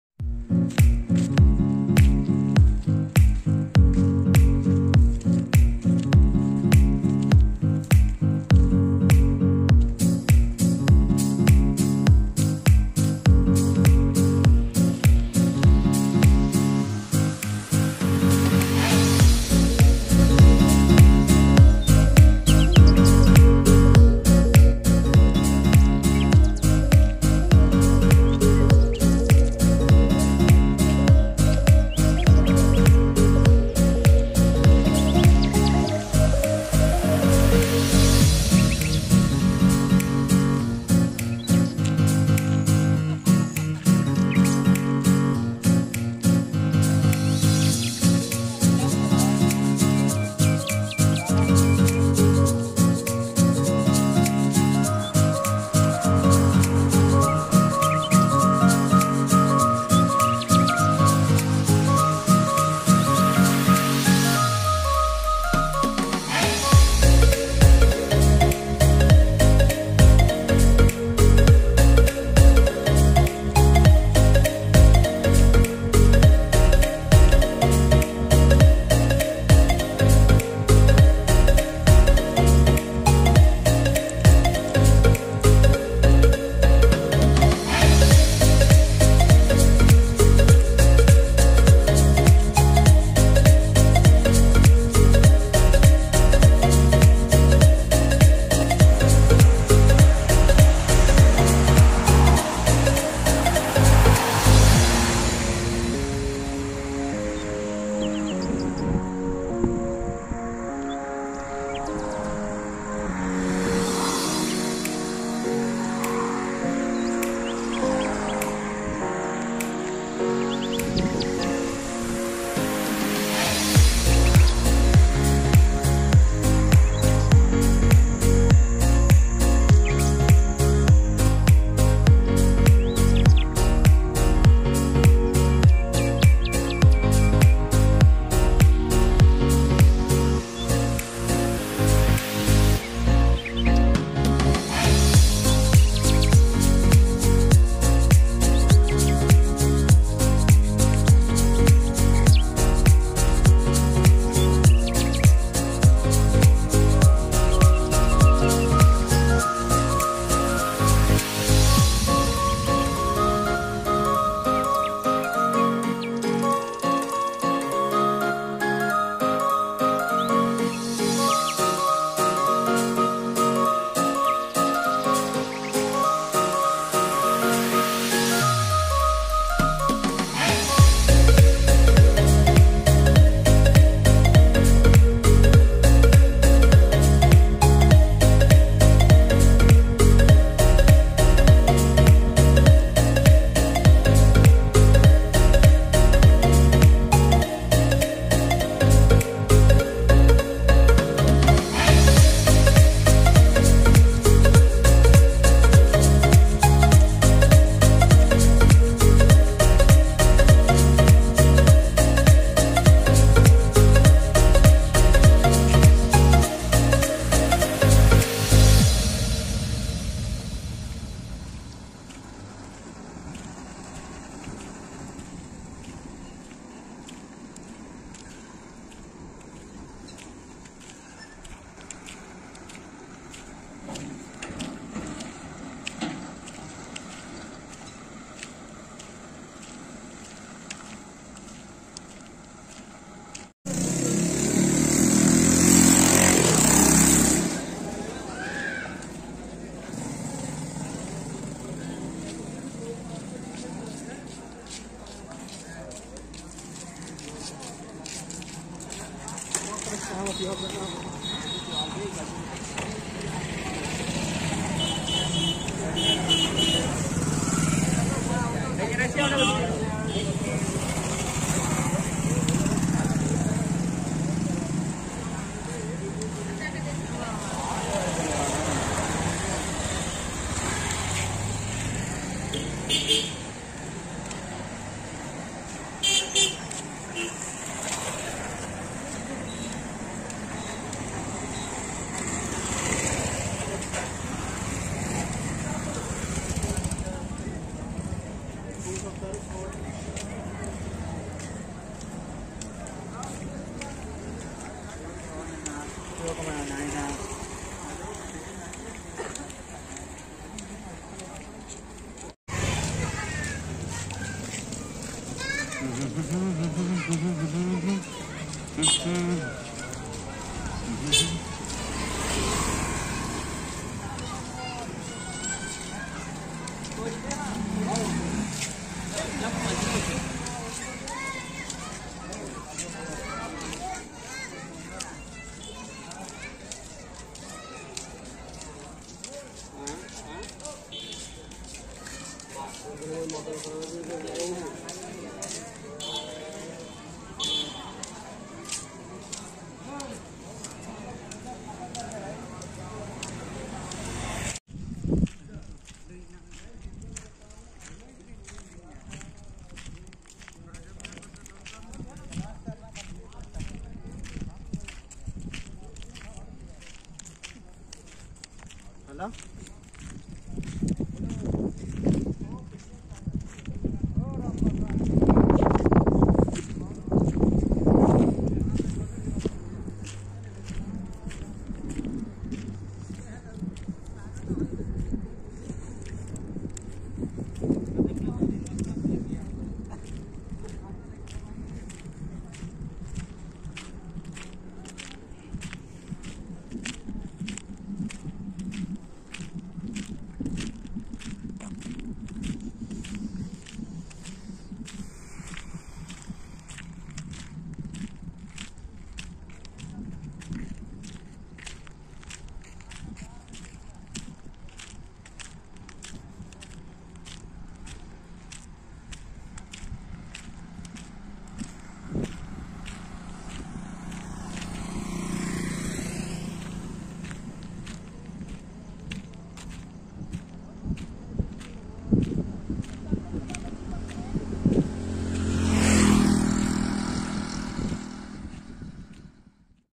7 March 2023
Rajiv Gandhi International airport construction work labour and office boy going to the market. go to the road and explore. nothing special Blog purpose.enjoy